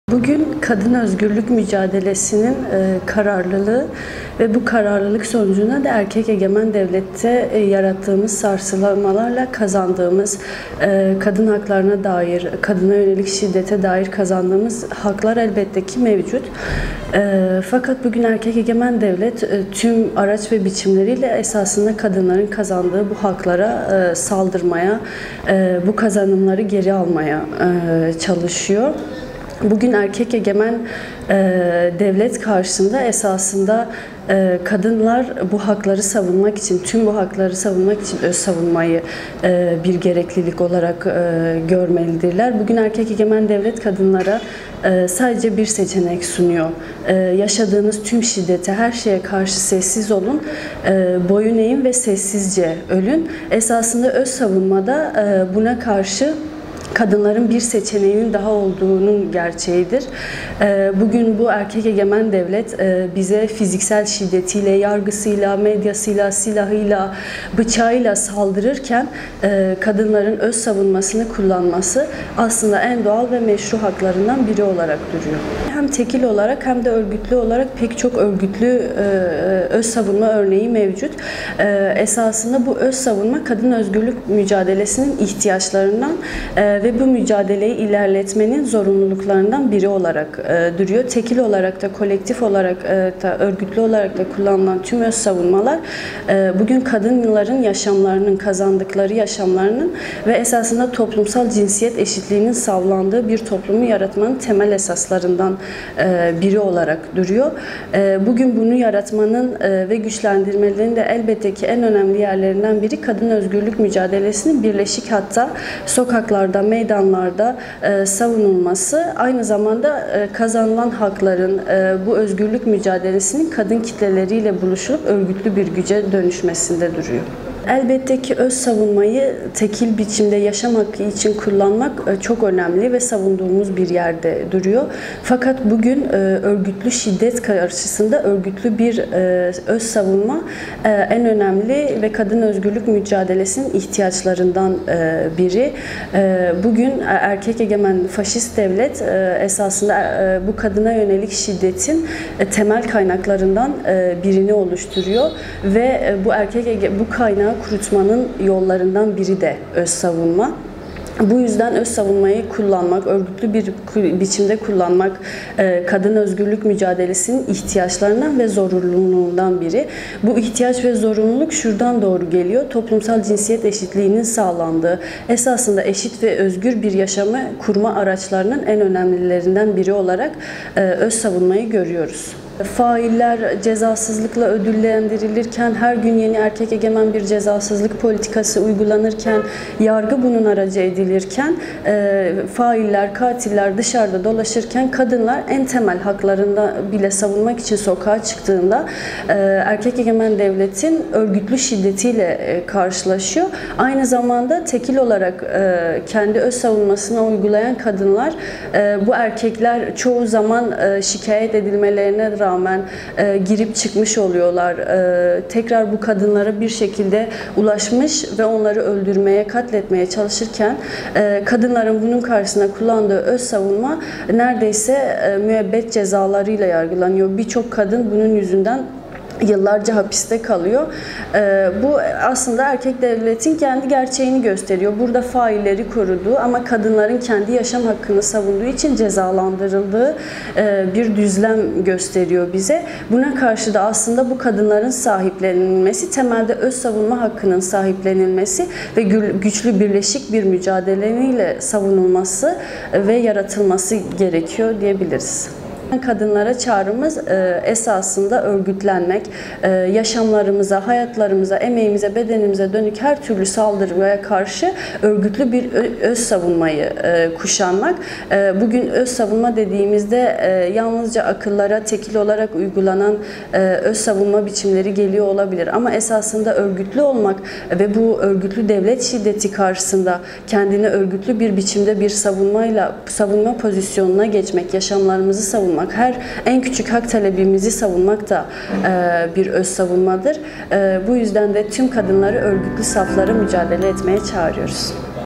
EN MEŞRU HAK: ÖZSAVUNMA
Erkek-devlet şiddetine karşı özsavunmanın en meşru hak olduğunu belirten SKM İstanbul Sözcüsü Leyla Can, "Özsavunma, var olan örgütlü şiddete karşı örgütlü bir savunma pozisyonuna geçmek, yaşamlarımızı savunmaktır” dedi.